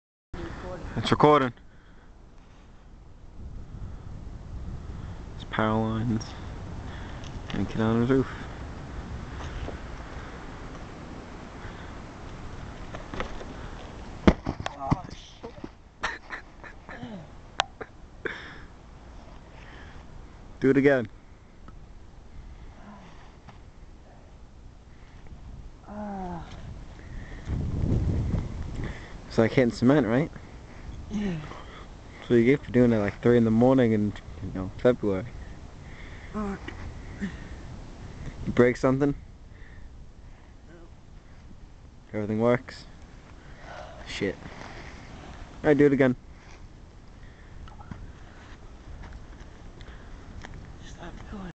Roof Jump
Jackass jumps off roof in cold at 3 in the morning and finds out hitting the ground is like hitting cement